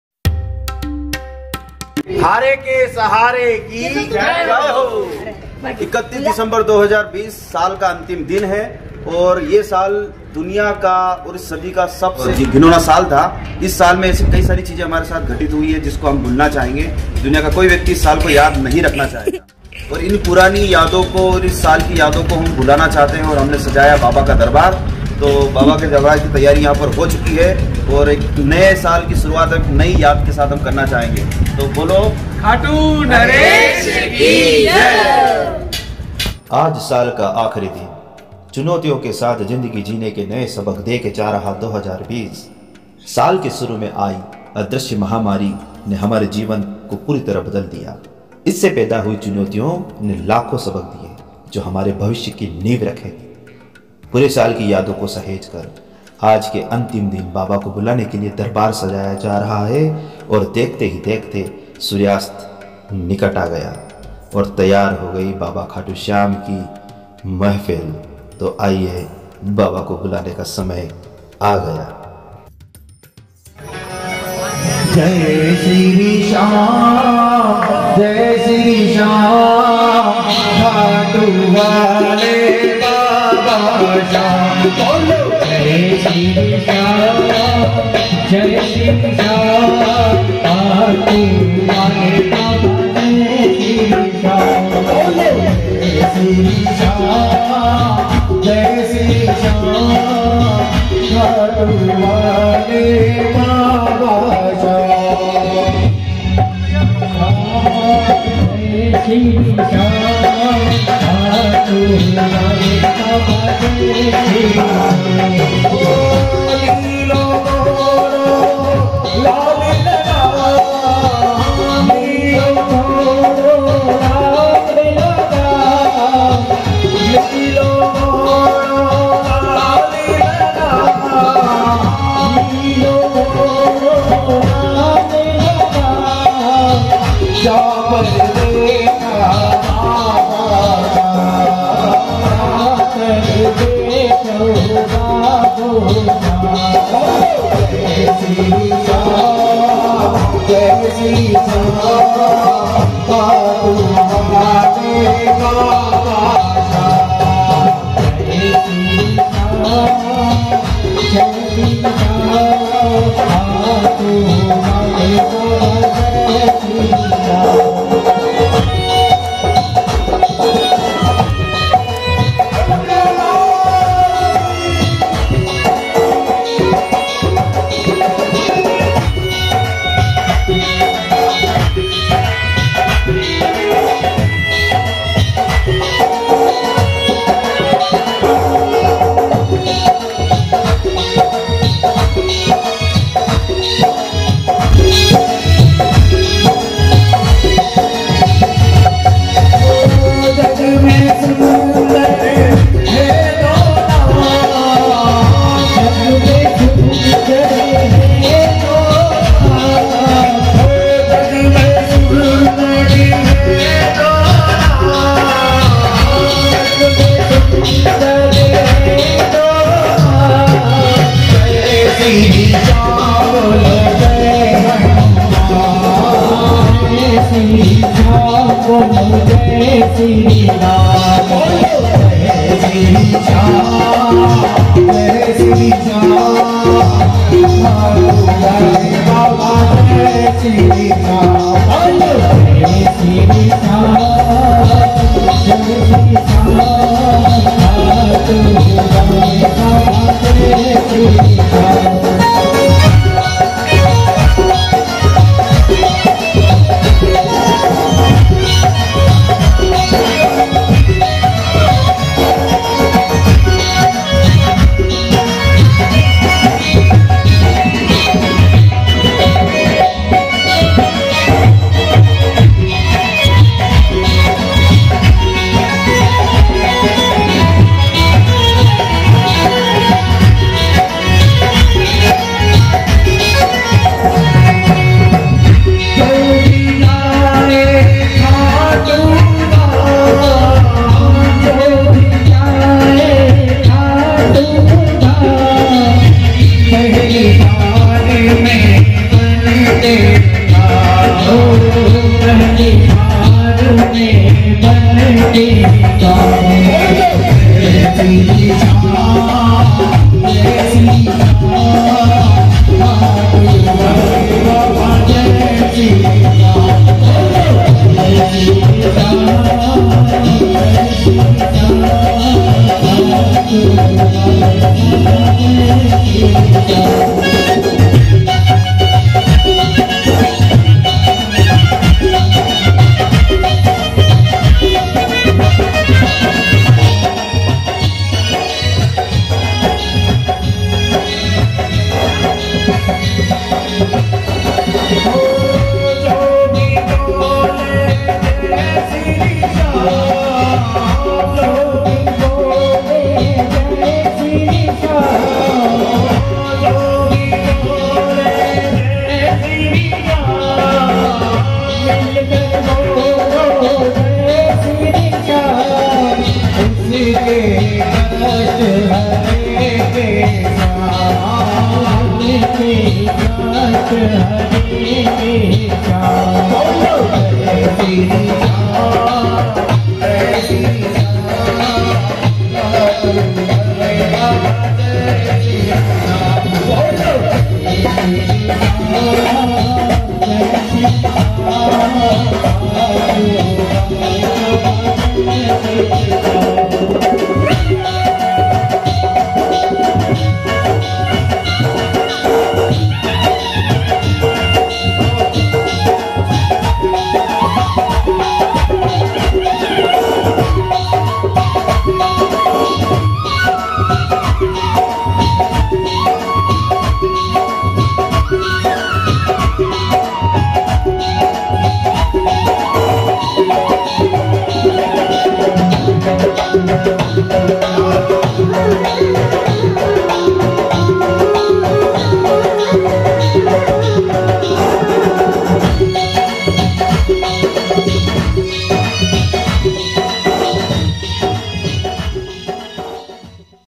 31 दिसंबर 2020 खाटू श्याम भजन संध्या सरवन
31 दिसंबर 2020 को सरवन में बाबा खाटू श्याम के दरबार को सजाया गया जहां श्याम के कीर्तन को करके भक्तों ने दरबार की रौनक बढ़ाई और बाबा के भजनों को गाकर साल के आखिरी दिन को एक यादगार दिन में बदला 2020 के ऐसी यादें जिनको भूलने के लिए बाबा श्याम को हमने याद किया और आने वाले साल में बाबा की याद बनी रहे ऐसी हम सब की आशा है